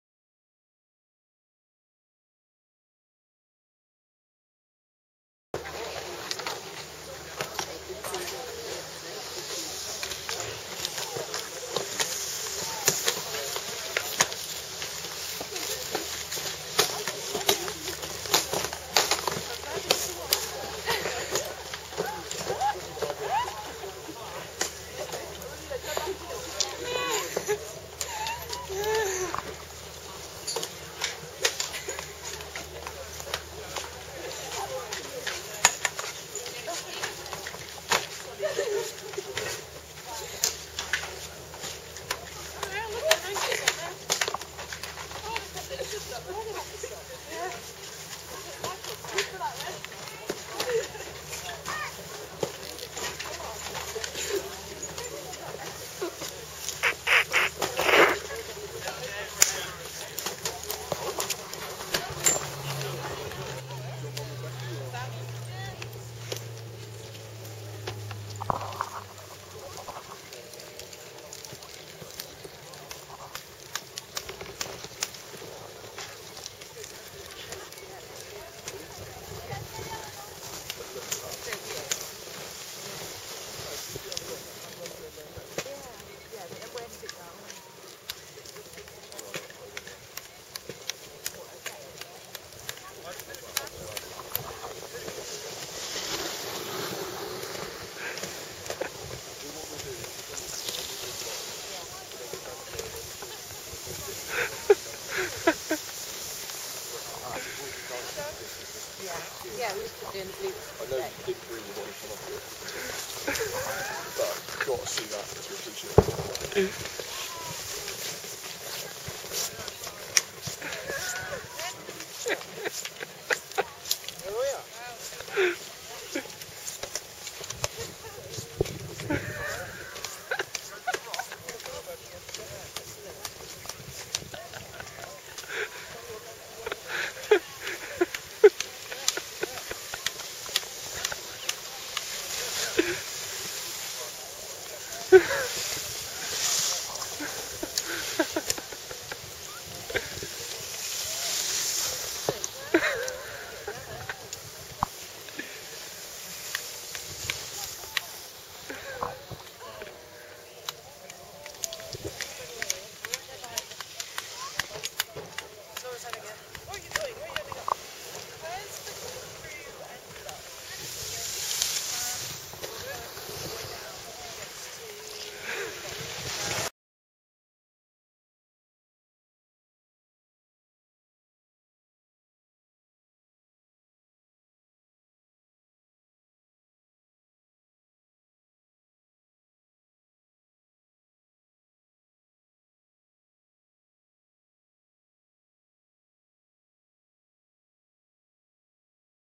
Two Funny Girls putting skis on in France
Two Funny Girls Putting skis on at Courchevel 1850in French Alps and Ski Snowboard Paintings by Pete Caswell at Courchevel & Meribel French Alps